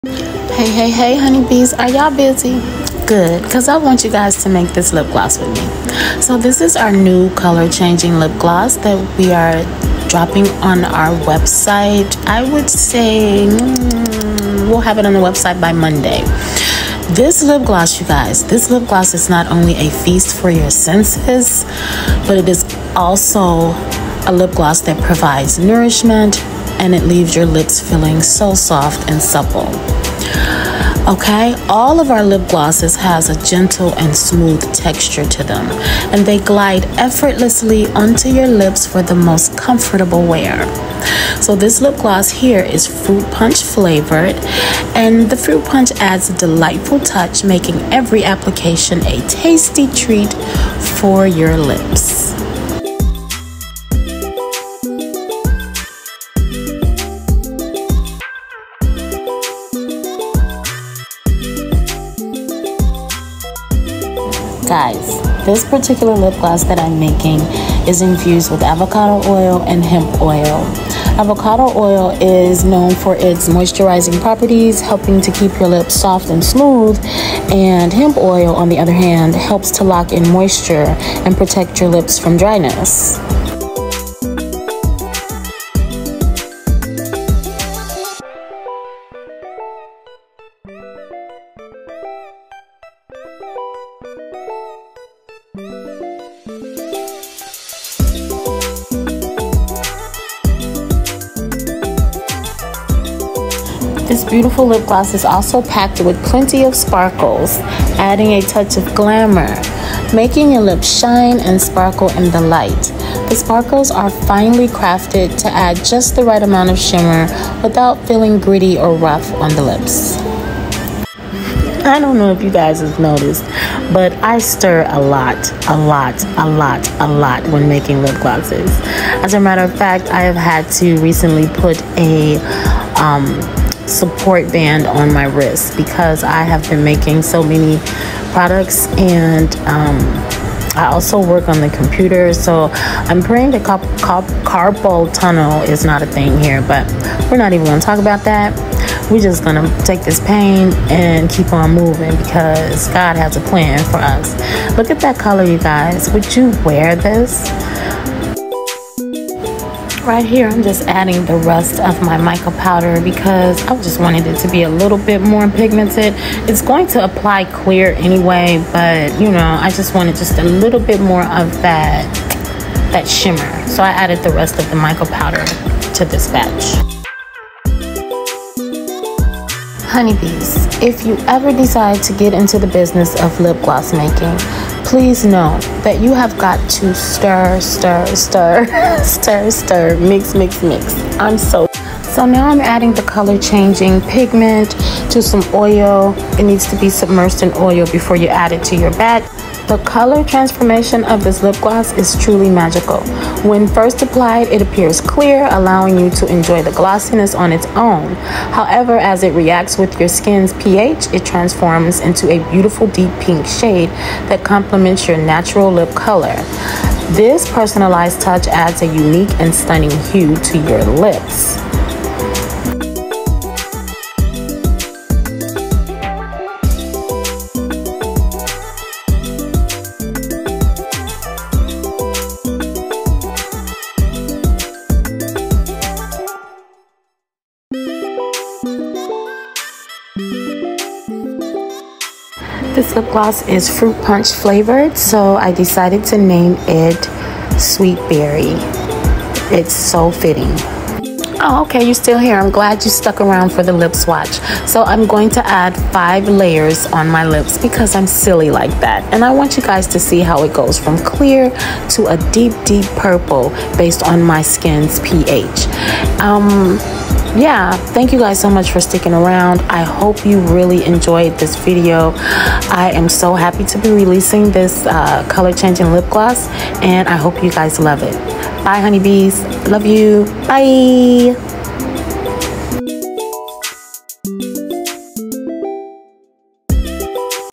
Let’s make a color changing lipgloss! What do you think? 🐝💛🐝🐝
Stay till the end for the swatch!!